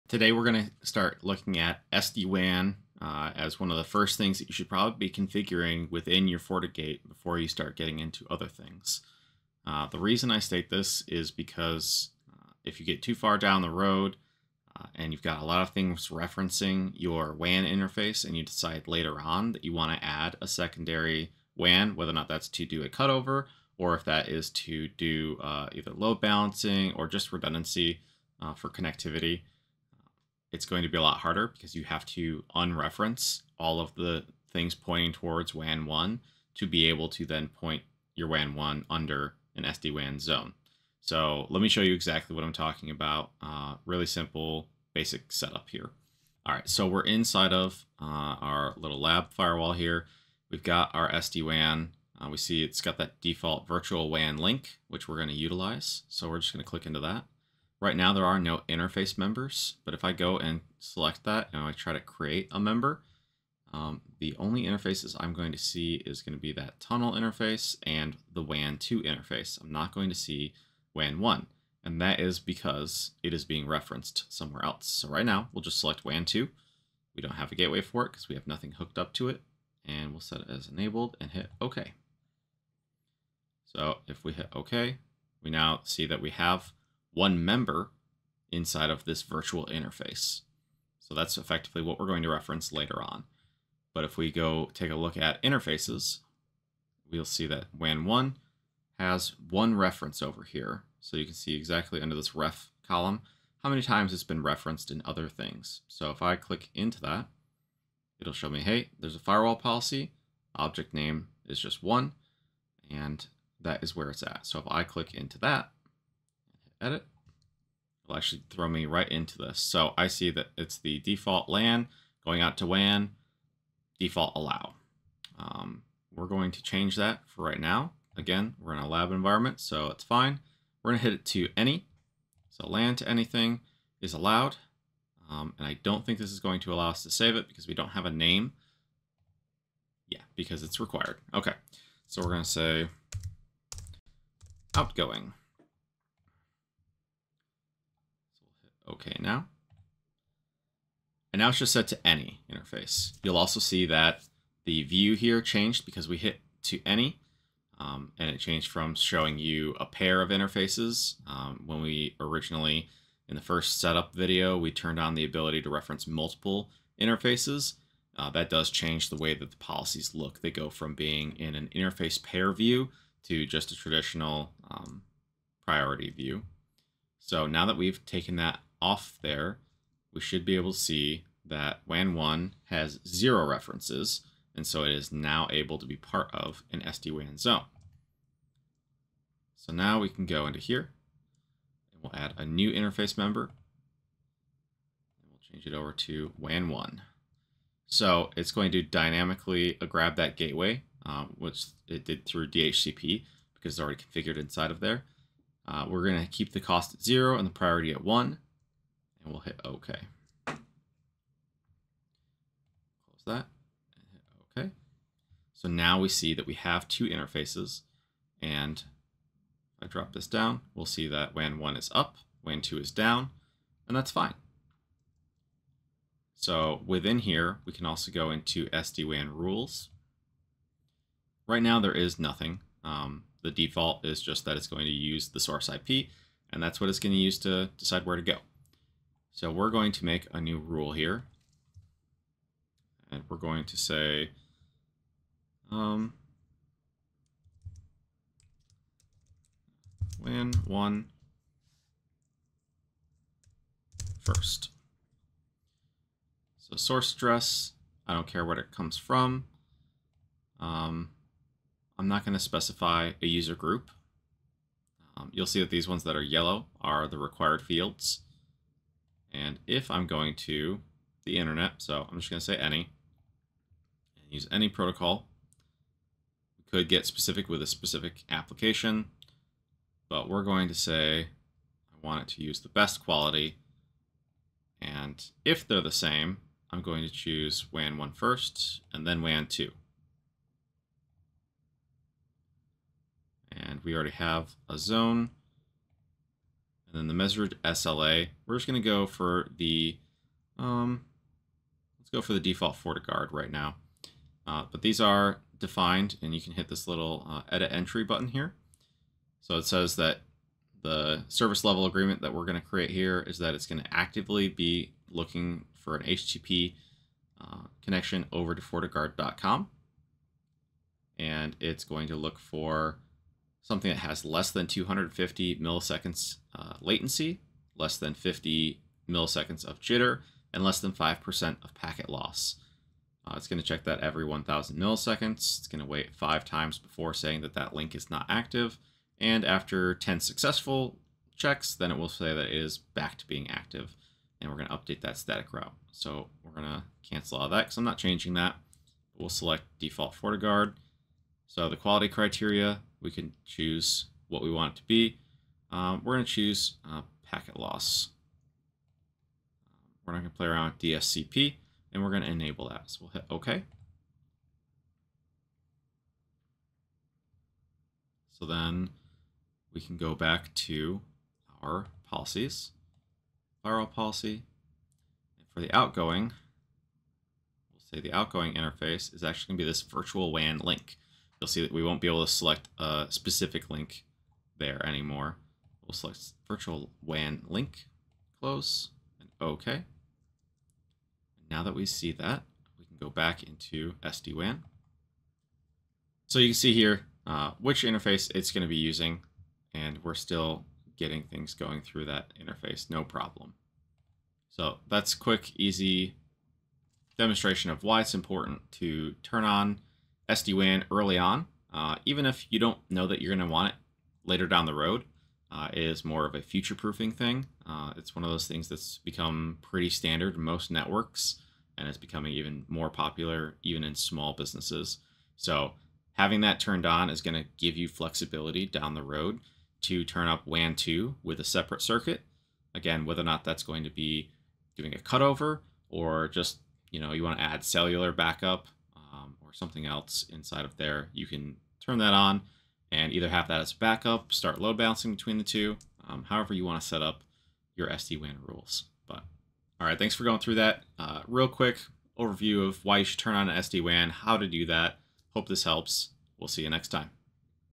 FortiGate Initial Setup - SD-WAN
Dive into the world of SD-WAN with me as we explore the critical initial steps of configuring SD-WAN on a FortiGate device. In this video, I'll share why it's essential to start with SD-WAN configurations before delving into more complex network setups. Join me in unlocking the potential of your network with SD-WAN!

Acronyms:
🔷 SD-WAN: Software-Defined Wide Area Network
🔷 WAN: Wide Area Network
🔷 LAN: Local Area Network